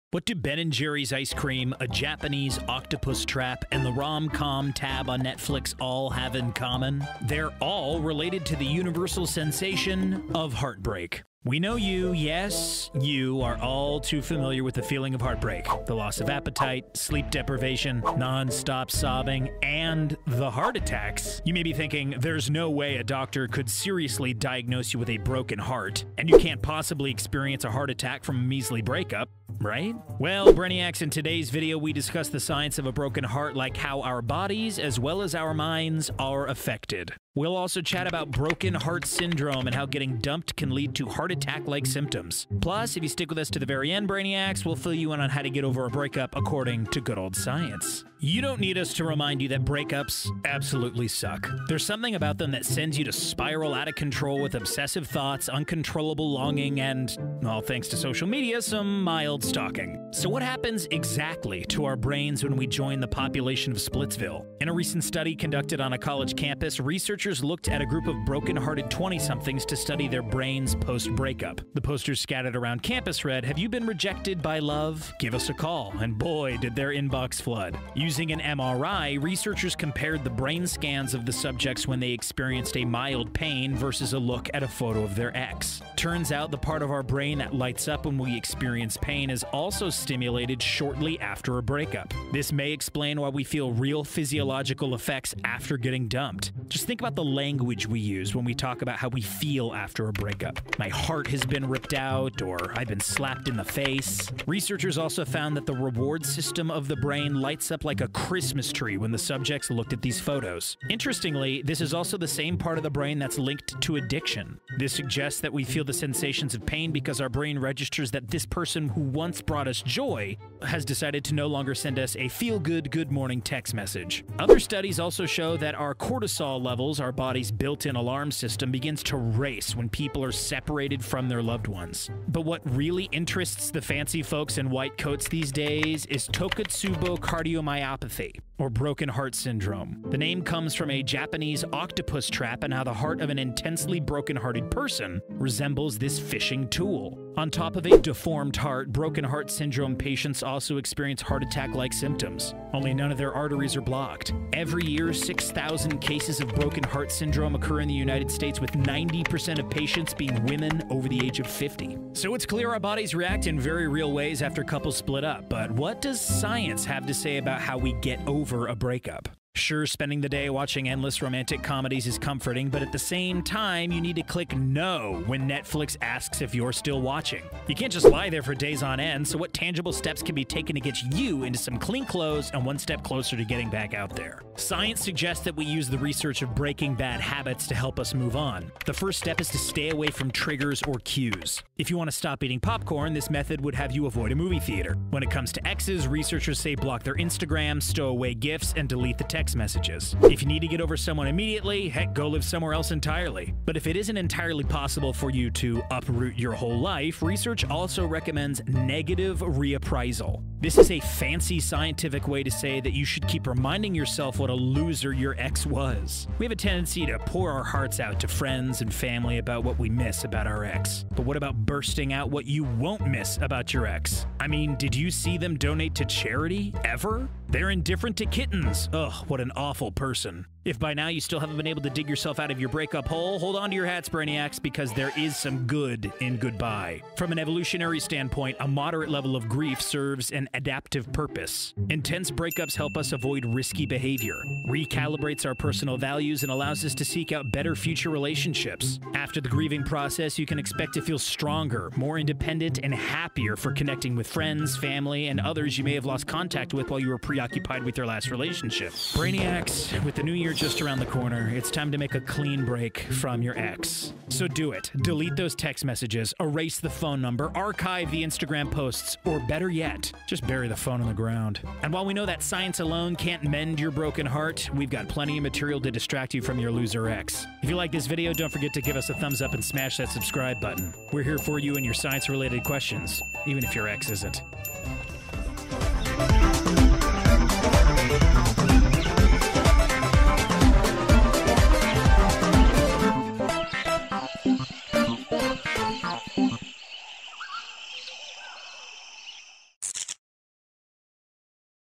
Can You Die From A Broken Heart?
Your poor heart goes through a lot. Not only does it pump blood through your body and beats throughout the day, every now and again, it goes through heartache. But a broken heart is just a metaphor...right? In today’s video, we answer that question...and the answer might surprise you. It turns out, there are real physiological effects to heart ache. We explain why you can literally feel like your heart is broken after an intense break up, plus, if you stick around to the end of the video, we give let you know the best way to get over a break up, according to science.